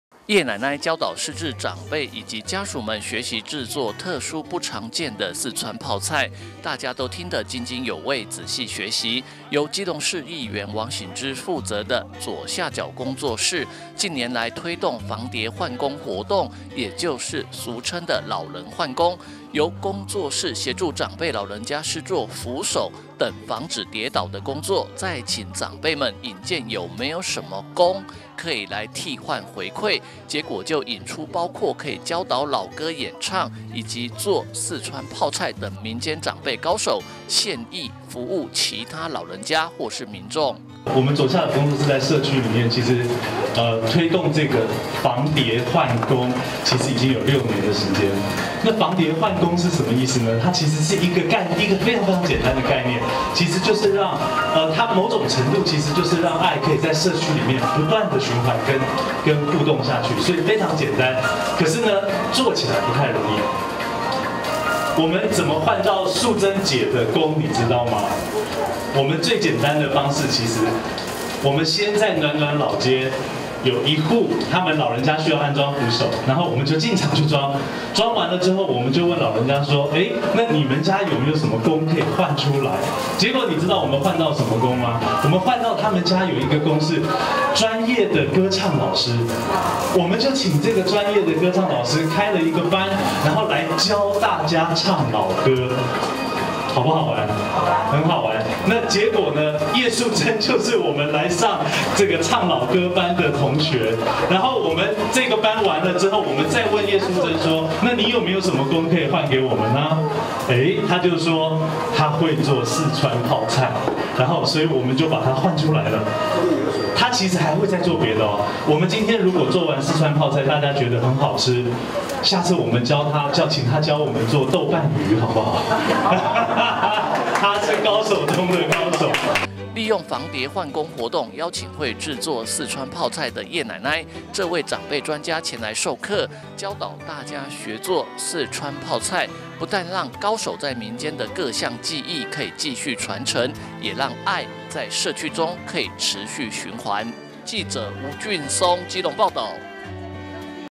新聞 基隆|暖暖左下角工作室|防跌換工|幫助老人家防止跌倒|請老人家教導傳授絕學|左下角「防跌換工」 引出民間高手獻藝回饋
左下角「防跌換工」 引出民間高手獻藝回饋

(中嘉吉隆新聞記者吳俊松/基隆報導)由基隆市議員王醒之負責的”左下角工作室”近年來推動”防跌換工”活動，也就是由工作室協助長輩老人家施做扶手等防止跌倒的工作，再請長輩引薦有沒有什麼”工”可以來替換回饋？結果就引出包括可以教導老歌演唱以及做四川泡菜等民間長輩高手獻藝服務，也讓愛在社區中循環！